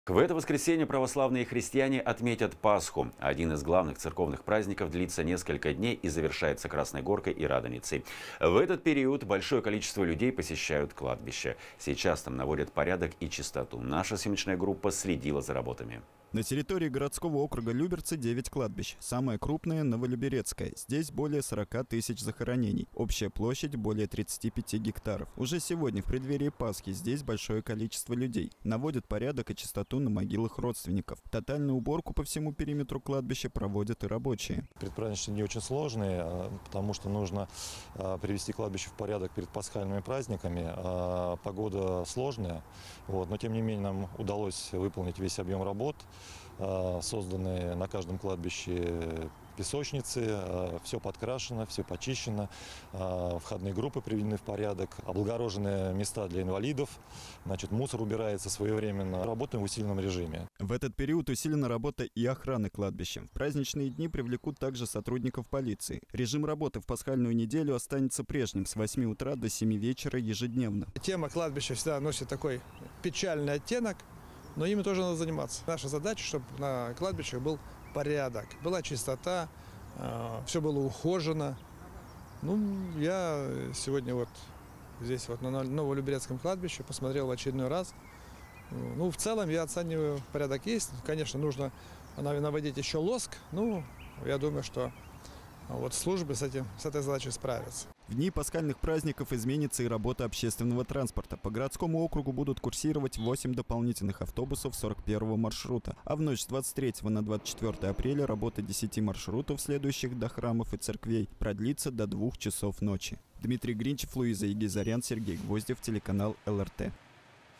Люберецкие кладбища готовят к Пасхе
В это воскресенье православные христиане отметят Пасху. Один из главных церковных праздников длится несколько дней и завершается Красной горкой и Радоницей. В этот период большое количество людей посещает кладбища. Сейчас там наводят порядок и чистоту. Наша съемочная группа следила за работами.

На территории городского округа Люберцы 9 кладбищ. Самое крупное – Новолюберецкое. Здесь более 40 тысяч захоронений. Общая площадь – более 35 гектаров. Уже сегодня, в преддверии Пасхи, здесь большое количество людей. Наводят порядок и чистоту на могилах родственников. Тотальную уборку по всему периметру кладбища проводят и рабочие.

Константин Новиков, руководитель Люберецкой ритуальной службы:
«Предпраздничные дни очень сложные, потому что нужно привести кладбища в порядок перед пасхальными праздниками. Погода сложная. Но, тем не менее, нам удалось выполнить весь объем работ, созданы на каждом кладбище песочницы, все подкрашено, все почищено, входные группы приведены в порядок, облагорожены места для инвалидов. Мусор убирается своевременно. Работаем в усиленном режиме».

В этот период усилена работа и охраны кладбища. В праздничные дни привлекут также сотрудников полиции. Режим работы в пасхальную неделю останется прежним – с 8 утра до 7 вечера ежедневно.

Владимир Ружицкий, глава г.о. Люберцы:
«Тема кладбища всегда носит такой печальный оттенок, но ими тоже надо заниматься. Наша задача, чтобы на кладбищах был порядок, была чистота, все было ухожено. Я сегодня на Новолюберецком кладбище посмотрел в очередной раз, в целом порядок есть. Надо наводить еще лоск, я думаю, что служба с этой задачей справится».

В дни пасхальных праздников изменится и работа общественного транспорта. По городскому округу будут курсировать 8 дополнительных автобусов 41-го маршрута. А в ночь с 23 на 24 апреля работа 10-ти маршрутов, следующих до храмов и церквей, продлится до 2-х часов ночи.